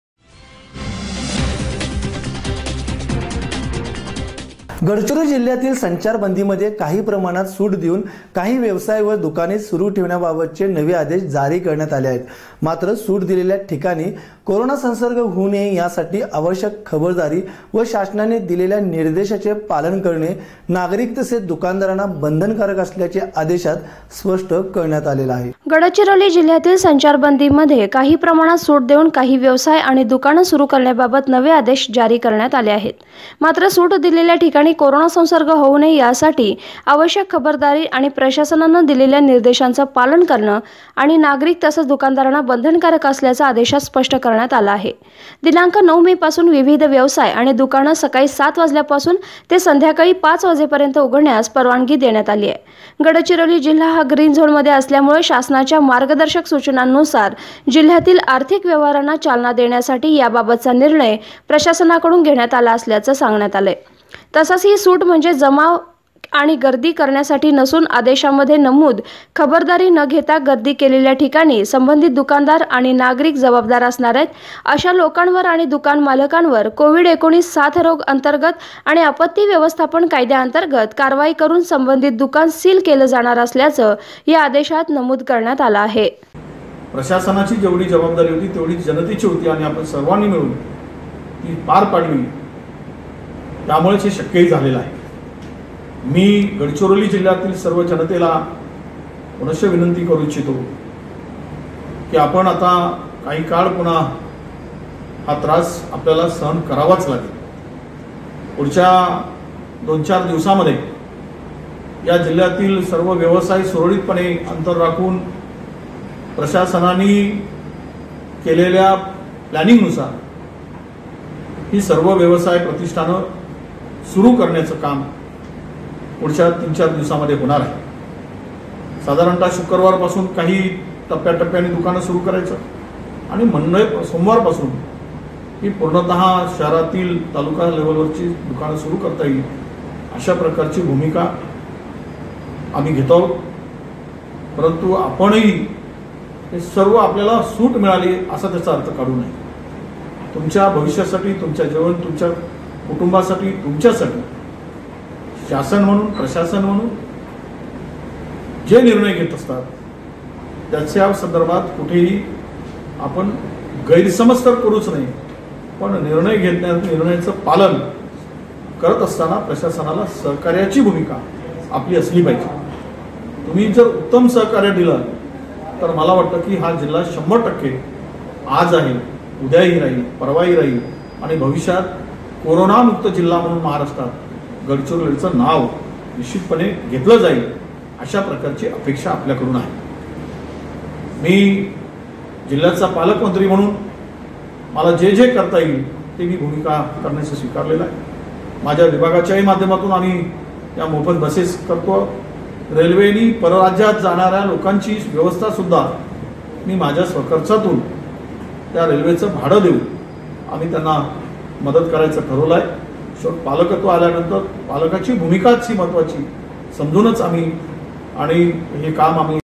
लॉकडाऊनमध्ये ग्रीन झोन असलेल्या गडचिरोली जिल्हयात व्यवसाय तसेच दुकाने सुरू करण्यास परवानगी
लॉकडाऊनमध्ये ग्रीन झोन असलेल्या गडचिरोली जिल्हयात व्यवसाय तसेच दुकाने सुरू करण्यास परवानगी, मात्र खबरदारी घेणे व निर्देशांचे पालन करणे बंधनकारक.@